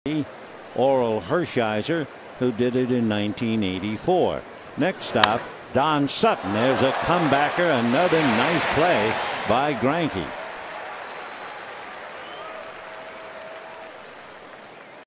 PHI@LAD: Greinke snares Gonzalez's comebacker
7/9/15: Zack Greinke shows off his quick reflexes by snaring a comebacker off the bat of Severino Gonzalez